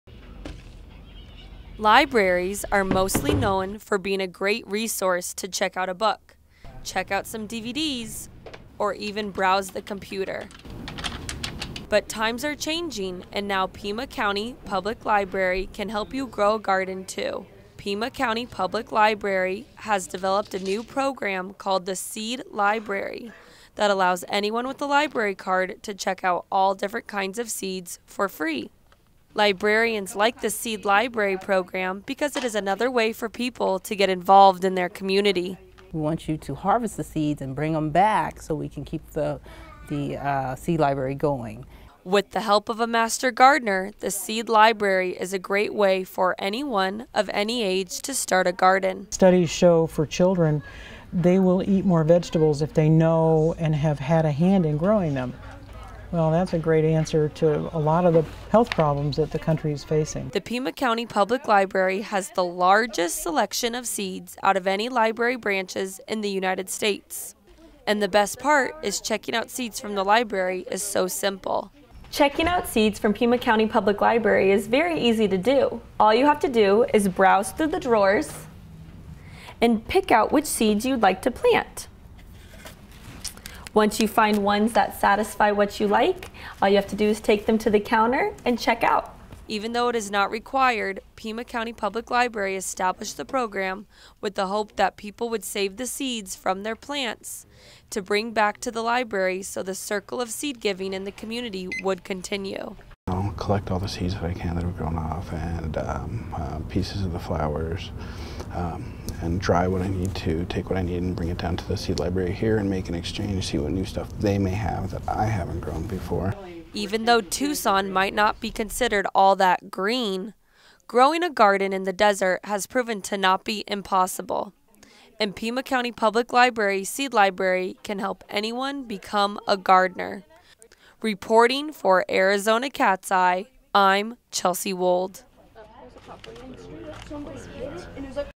Seed Library
Pima County Public Libraries are not just for books any more.  Arizona Cat's Eye Reporter Chelsea Wold shows us how Pima County Public Libraries is growing in a greener direction.